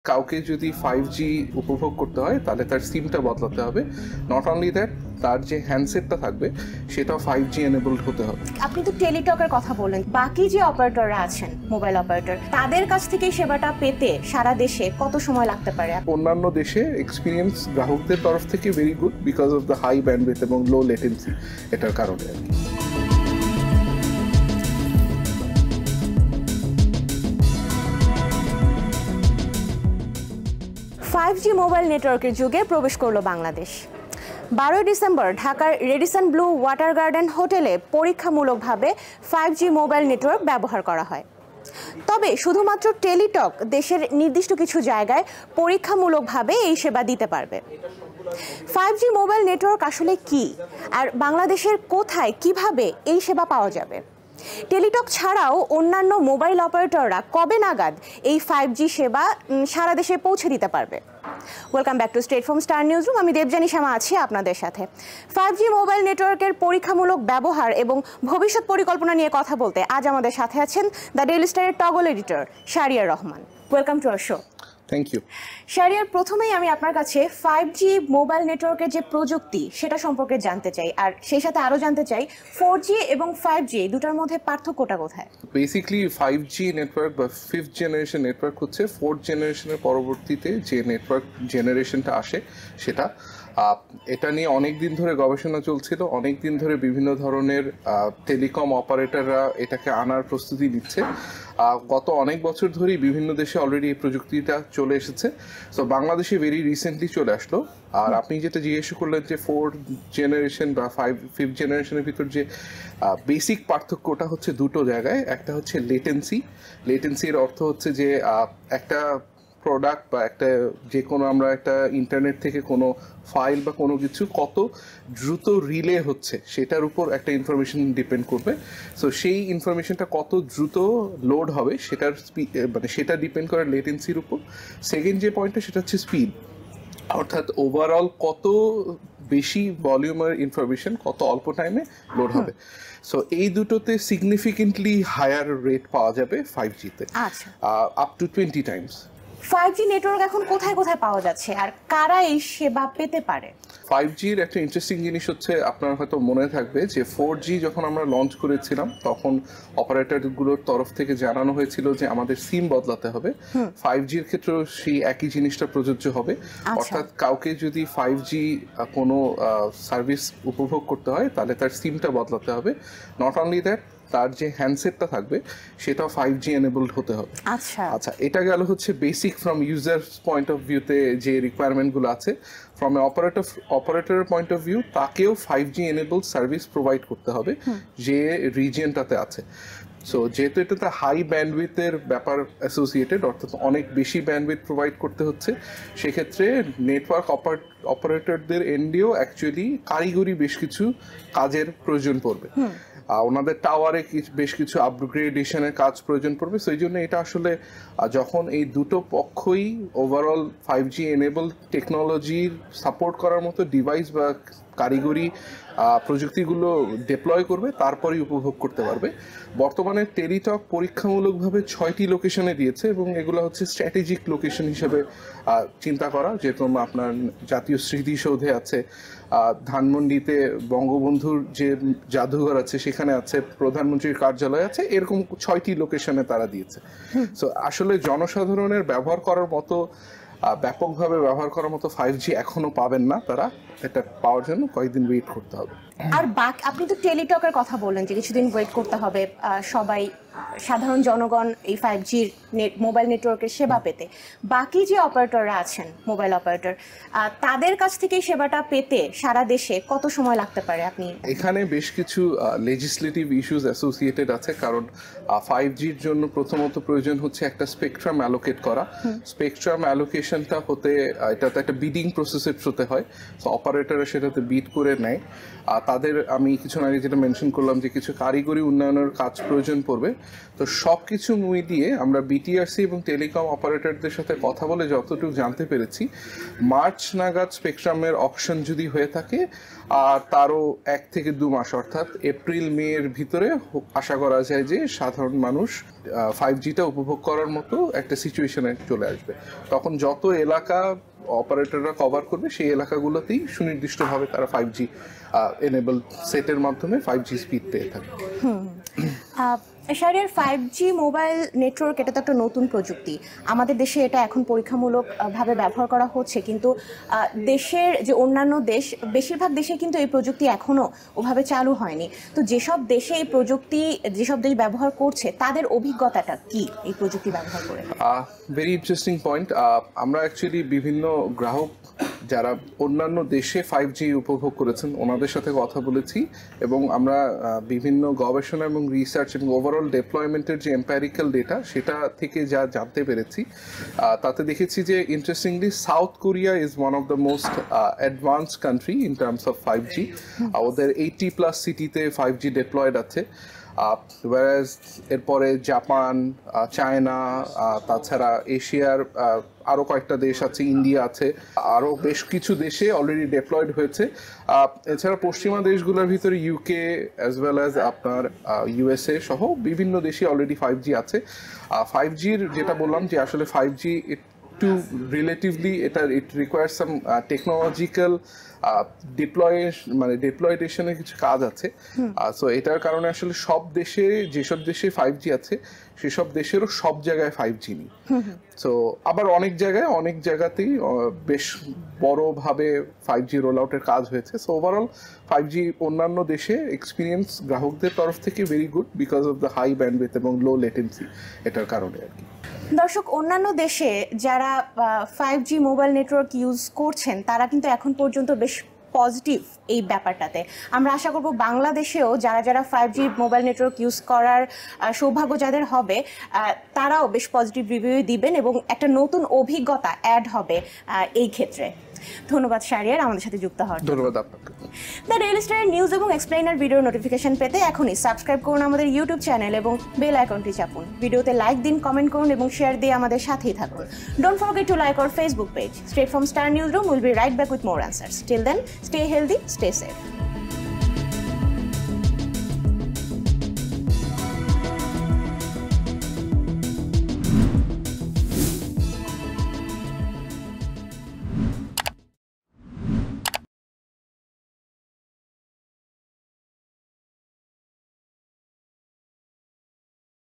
ফাইভ-জি যুগে বাংলাদেশের প্রবেশ
ফাইভ-জি মোবাইল নেটওয়ার্কের যুগে প্রবেশ করল বাংলাদেশ। গত ১২ ডিসেম্বর ঢাকার রেডিসন ব্লু ওয়াটার গার্ডেন হোটেলে পরীক্ষামূলকভাবে ফাইভ-জি নেটওয়ার্ক ব্যবহার করা হয়। তবে, শুধু টেলিটক নির্দিষ্ট কিছু জায়গায় এ সেবা দিবে।

ফাইভ-জি মোবাইল নেটওয়ার্ক আসলে কী? বাংলাদেশে কোথায়, কীভাবে ফাইভ-জি ব্যবহার করা যাবে? টেলিটক ছাড়া অন্য  অপারেটররা কবে ফাইভ-জি সেবা পৌঁছে দিতে পারবে সারা দেশে?

ফাইভ-জি মোবাইল নেটওয়ার্ক নিয়ে আজকের স্ট্রেইট ফ্রম স্টার নিউজরুমে দেবযানী শ্যামা আছেন দ্য ডেইলি স্টারের টগল এডিটর শাহরিয়ার রহমানের সঙ্গে।